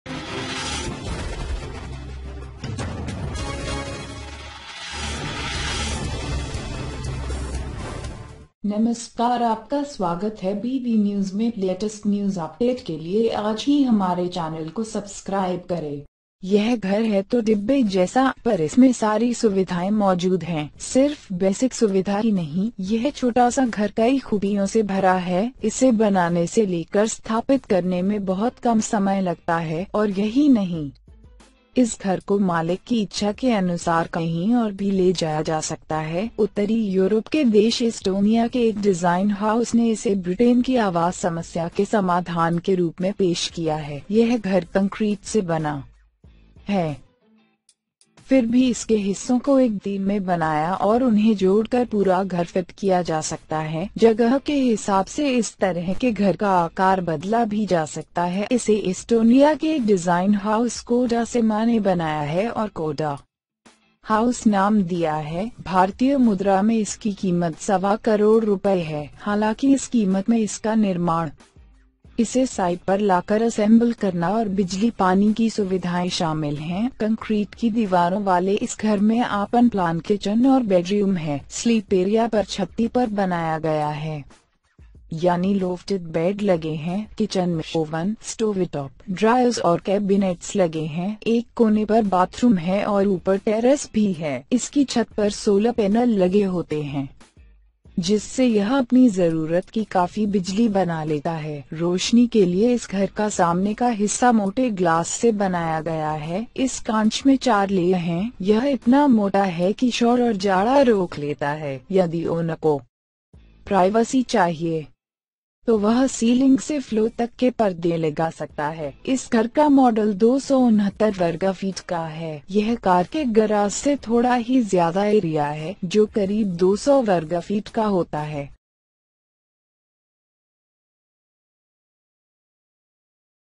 इस डिब्बे जैसे घर की कीमत है सवा करोड़ रुपए, मौजूद हैं सारी सुविधाएं!
It is a house, like a cans, but it has all the facilities in it. Not just the basic feature, this small house is full of many features. It takes a lot of time to make it from setting up. And not only this, this house can be moved somewhere as per the wishes of the owner. A design house in the country of Northern Europe Estonia has presented it as a solution to the UK housing problem.

This house is made of concrete, yet its parts are made in one day and can be fitted to the whole house by adding them. According to the place, the size of this house can also be changed. It has been made by Estonia's design house, Kodasema, and has named the Koda House. Its value is Rs. 50 crores in Indian currency. Although this price is built on it, it is brought to the site and assembling of power and water facilities. In this concrete walled house, there is an oven plan kitchen and bedroom. Sleep area is built on the cast, i.e. looped beds. The oven, the stovetop, the drawers and the cabinets are in the kitchen.

There is a bathroom on one corner and there is a terrace above. Its roof is mounted on a solar panel, making it sufficient power of its own needs. The front part of this house has been made from thick glass for the illumination. There are four layers in this glass. It is so thick that it stops the noise and the waist. If the owner wants privacy, then he can screen the ceiling from the floor. The model of this house is 269 square feet. It is a bit more area than the car garage, which is about 200 square feet.

BB NEWS !!